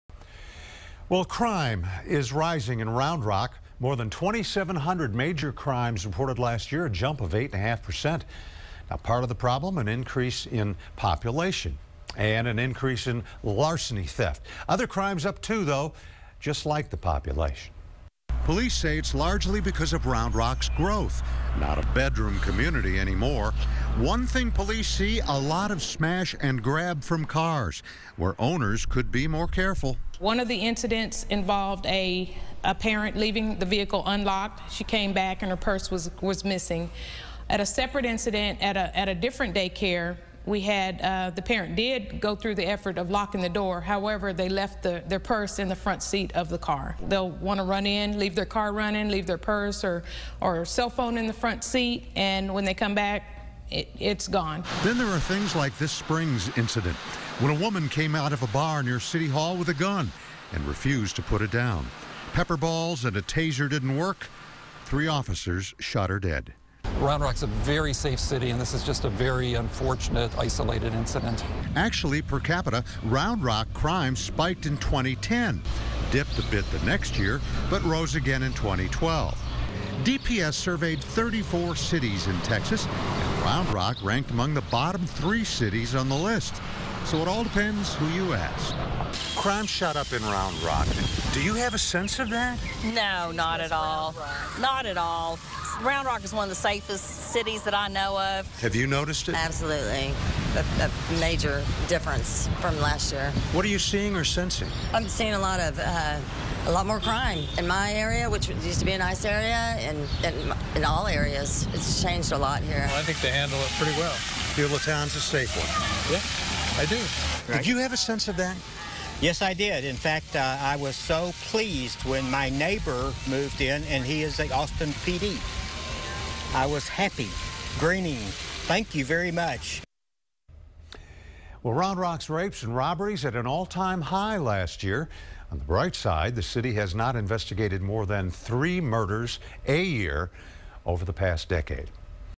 Rock Rock sees increase in crime
But the fast-growing Austin suburb remains comparatively safe.